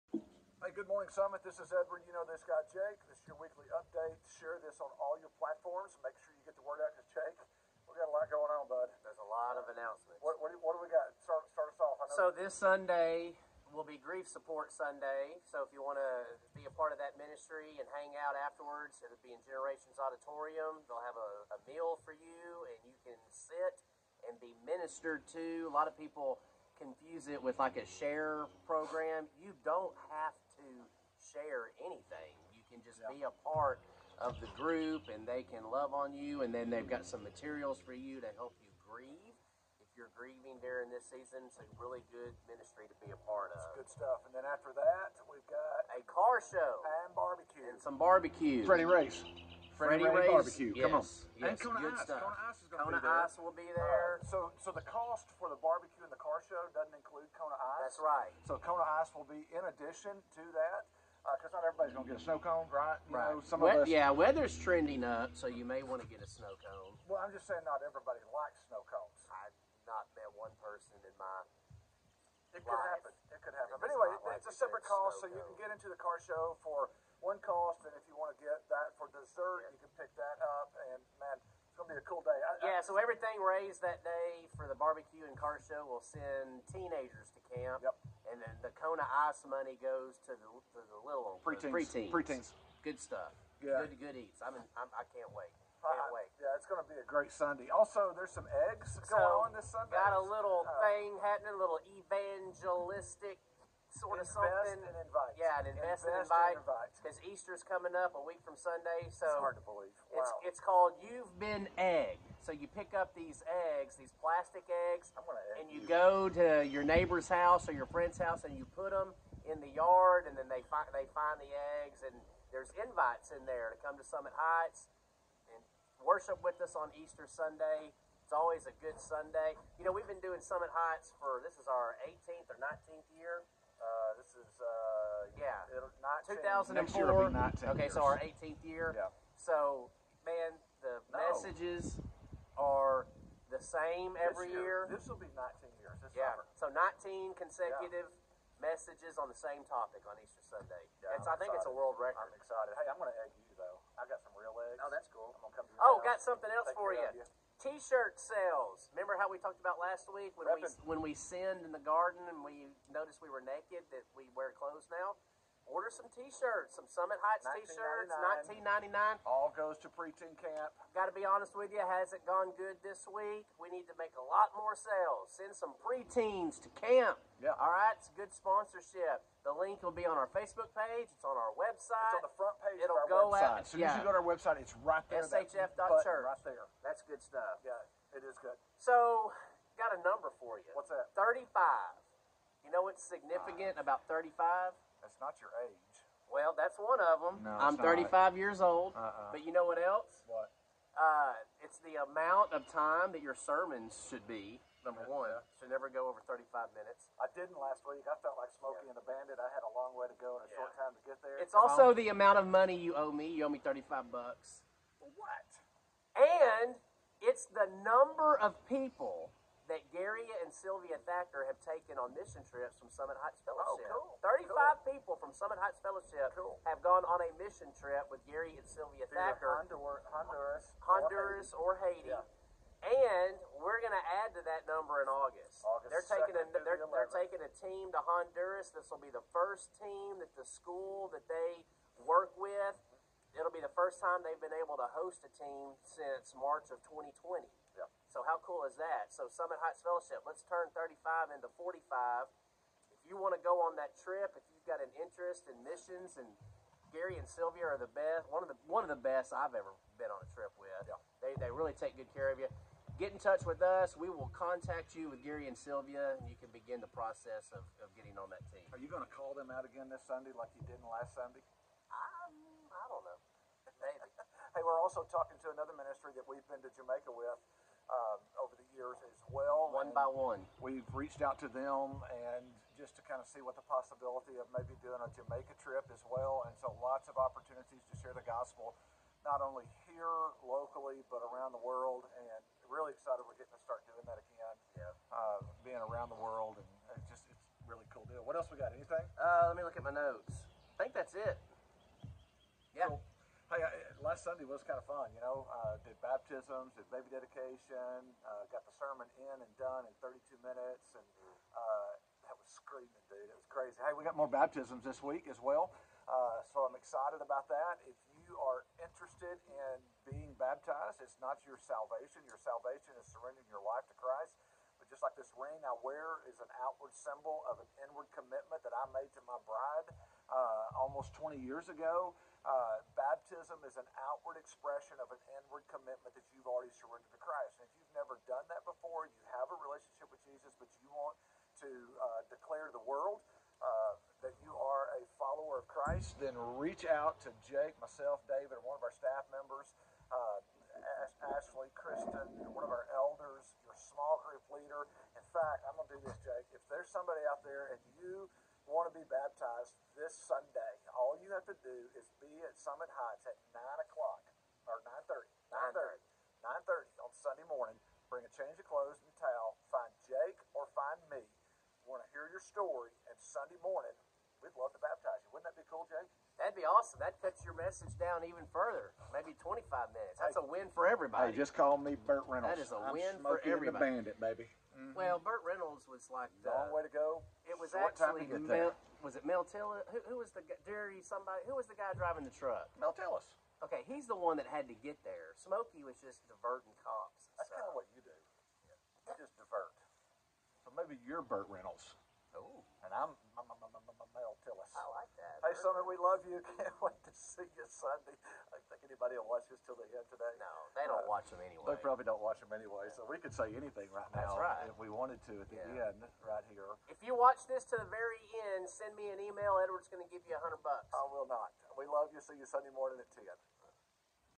Weekly Update 4-7-22
Grief Support, BBQ - CAR SHOW, The number 35 and a Smoky and the Bandit reference!